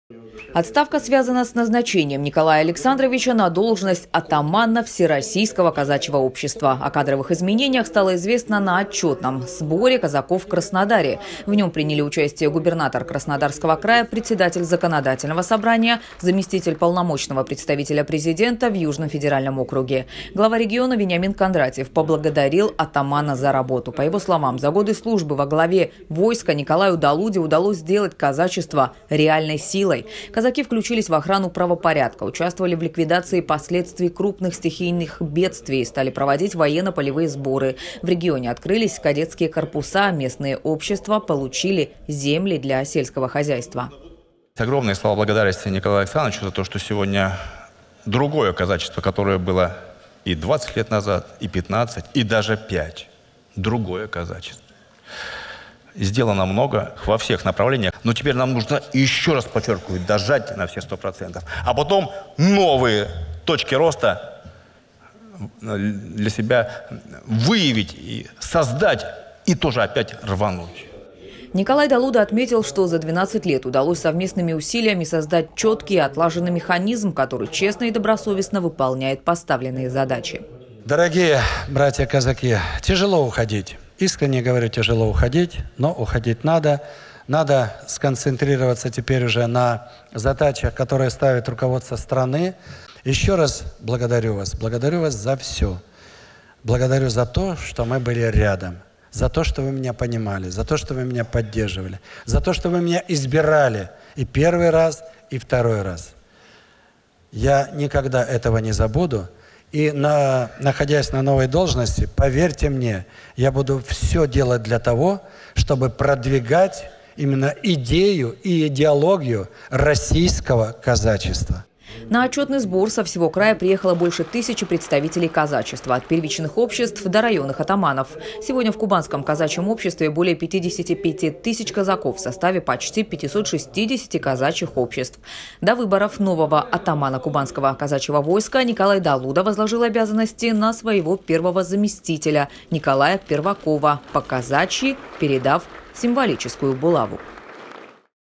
Николай Долуда покинул пост вице-губернатора Кубани. Новости Эфкате Сочи
Распоряжение об отставке подписал губернатор Краснодарского края. Кроме того, Долуда сложил с себя полномочия атамана Кубанского войска. Отставка связана с его назначением на должность атамана Всероссийского казачьего общества.

О кадровых изменениях стало известно на отчётном сборе казаков в Краснодаре. Глава региона Вениамин Кондратьев поблагодарил атамана за работу. По его словам, за годы службы во главе войска Николаю Долуде  удалось сделать казачество реальной силой. Казаки включились в охрану правопорядка, участвовали в ликвидации последствий крупных стихийных бедствий, стали проводить военно-полевые сборы. В регионе открылись кадетские корпуса, местные общества получили земли для сельского хозяйства.